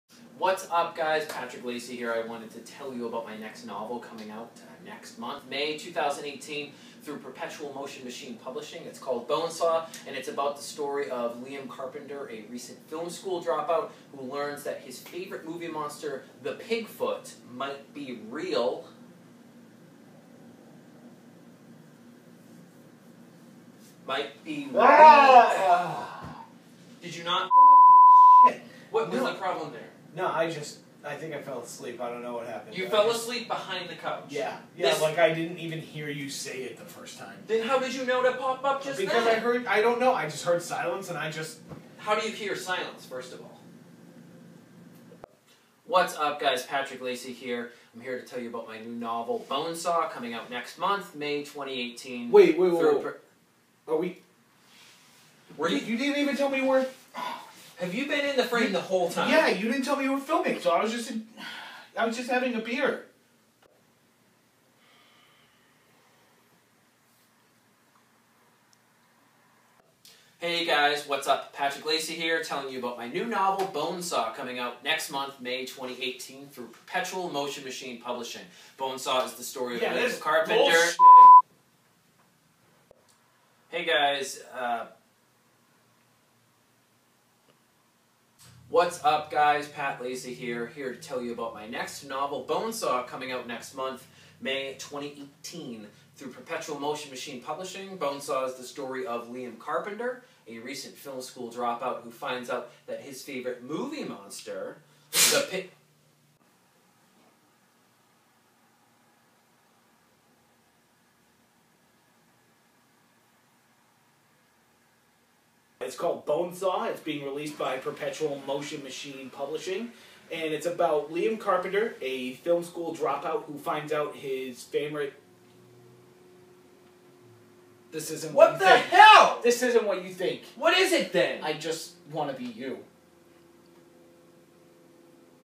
Bone Saw by Patrick Lacey (Book Promo)
I recently sat down to create this promo for my new novel BONE SAW but ran into some unexpected difficulties with a hairy man wearing a pig mask. Pre-order the novel now